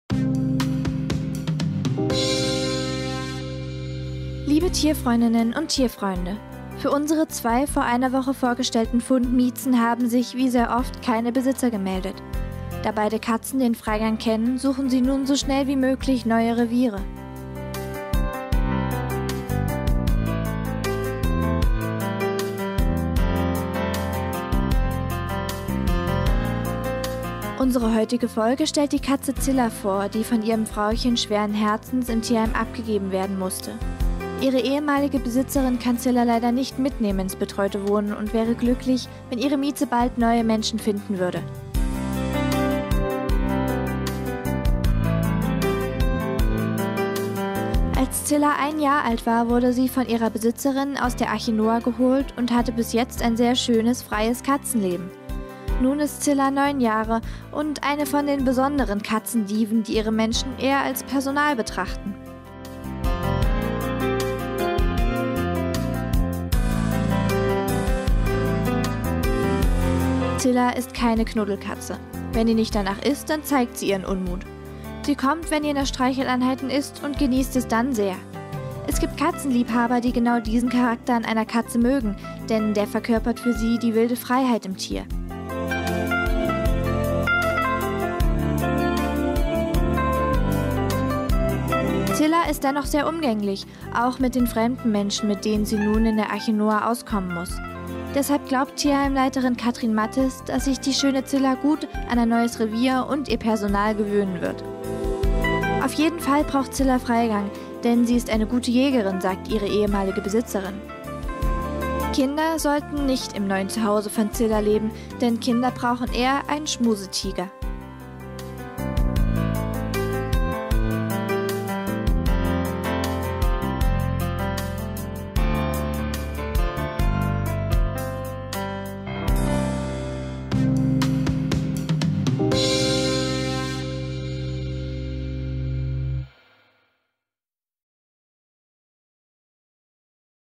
Cilla, eine wahre Katzendiva
Heute präsentieren wir die schöne Katze Cilla, die von ihrem Frauchen schweren Herzens im Tierheim abgegeben werden musste. Cilla ist keine generelle Schmusekatze, sondern zeigt genau, wann ihr zum Schmusen ist und wann nicht. So mancher Katzenfreund liebt gerade diesen Charakterzug.  Mehr Informationen im Tierheim Gotha unter Tel. 03621 / 755 425.